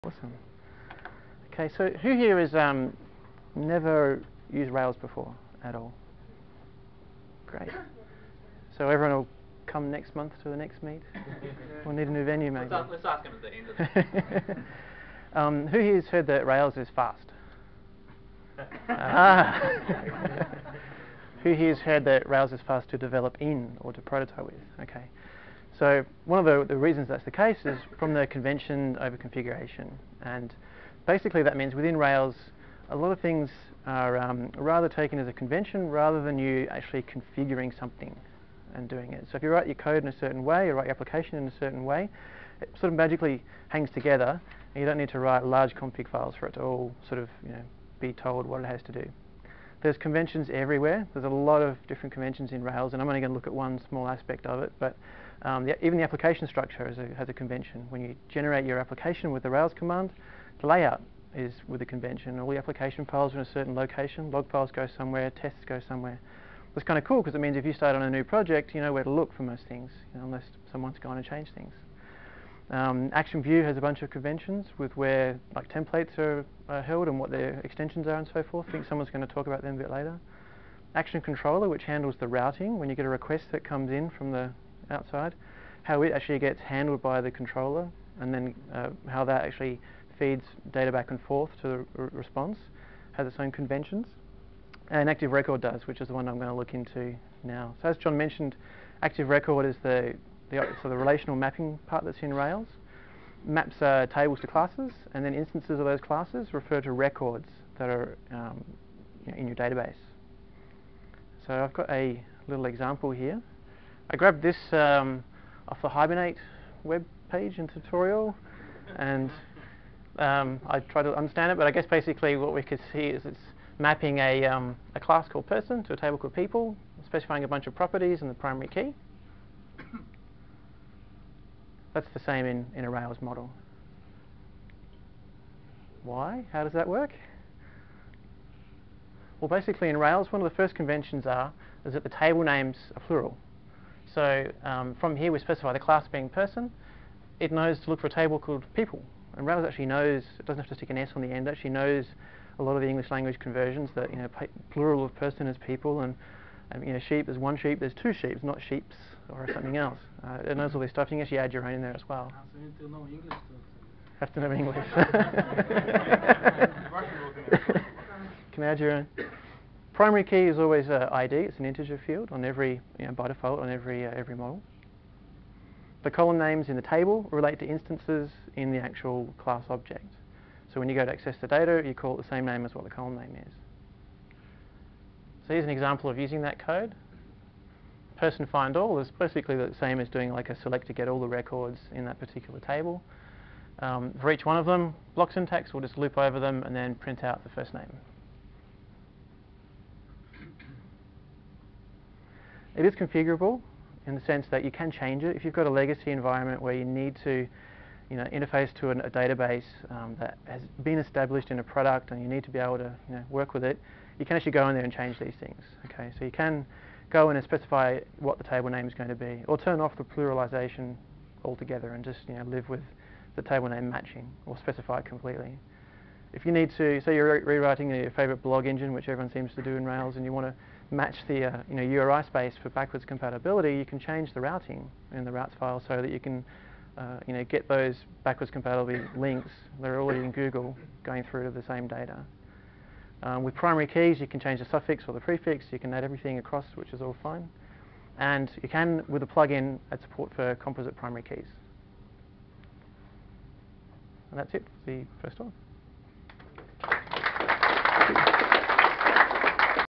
Rails: Convention over Configuration
Presentation about Rails use of convention over configuration, given at the Ruby Nuby Night at the Melbourne Ruby/Rails User Group.